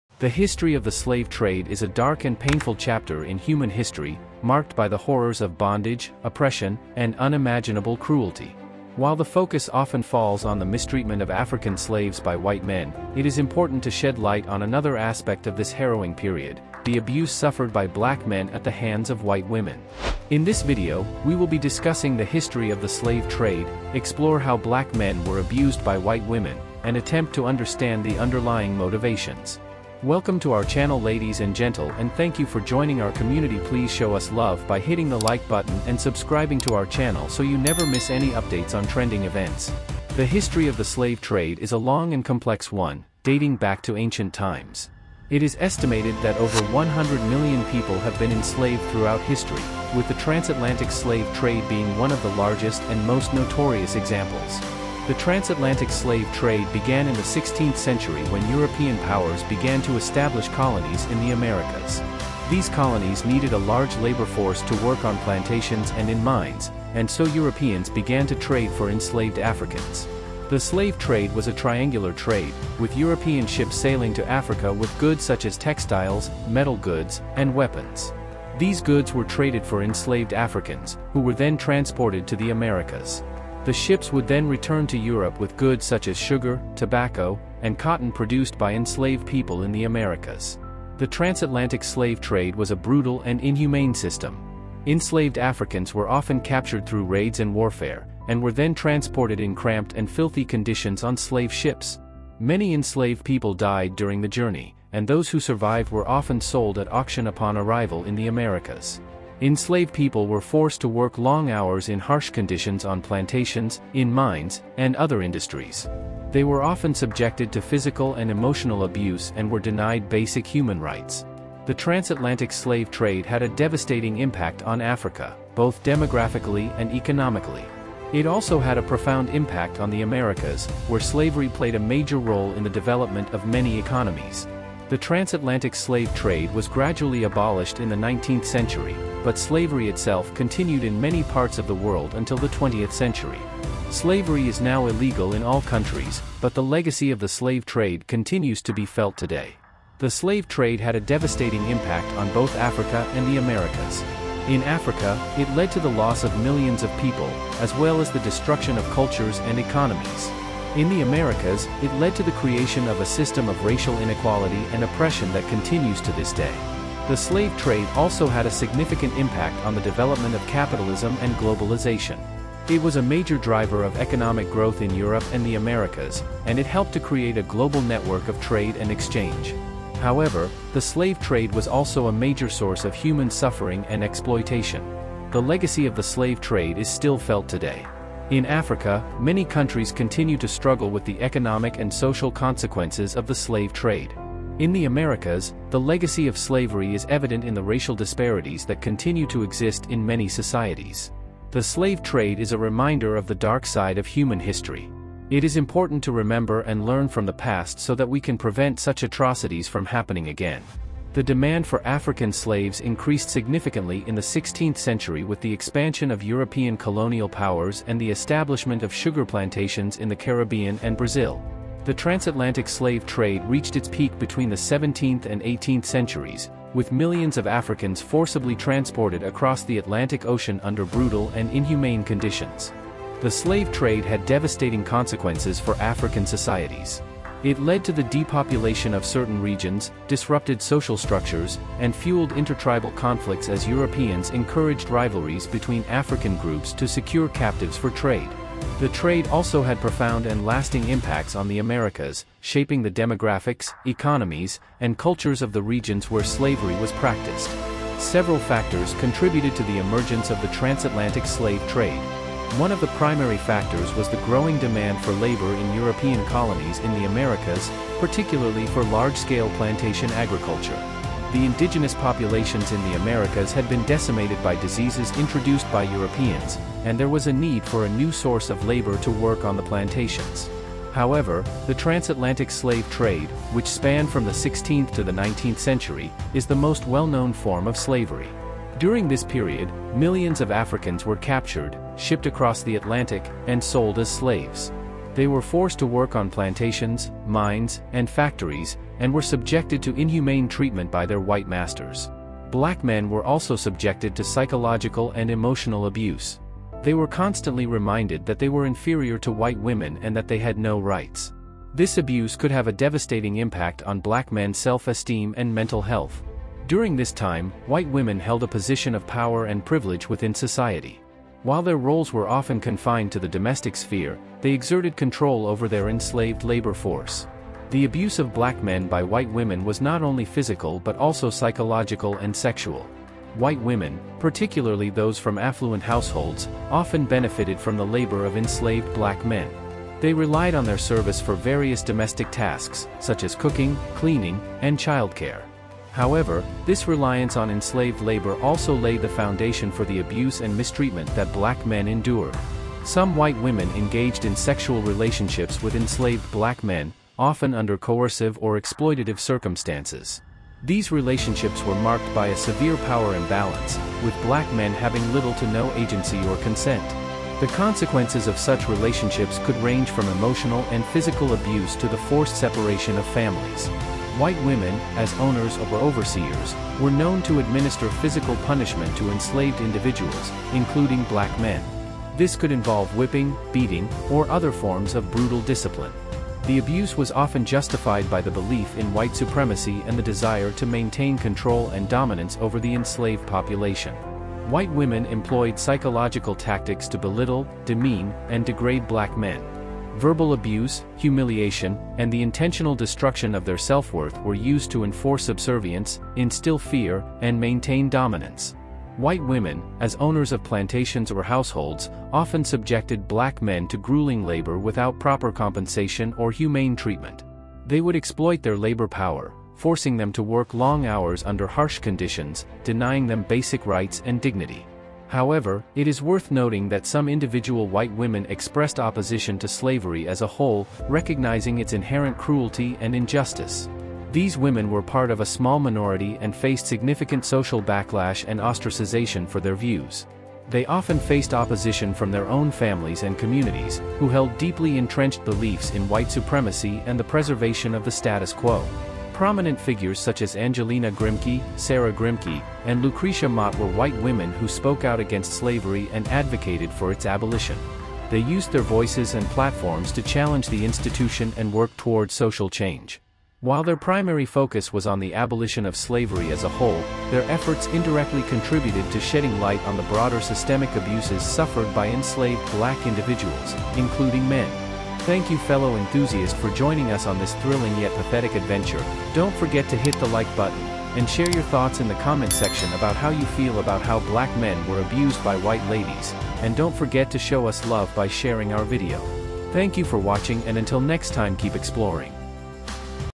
How Black Males Were Abused By White Females
The history of the slave trade is a dark and painful chapter in human history, marked by the horrors of bondage, oppression, and unimaginable cruelty. While the focus often falls on the mistreatment of African slaves by white men, it is important to shed light on another aspect of this harrowing period – the abuse suffered by black men at the hands of white women.

Transcript